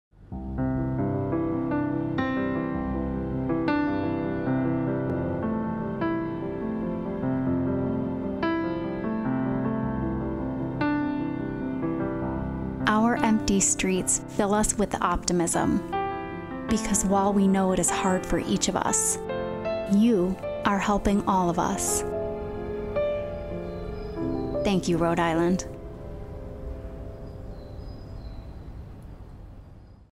"Empty Streets" Lifespan
A tribute to the people of Rhode Island who made countless sacrifices to help the health care workers on the frontlines of the pandemic that threatened us all.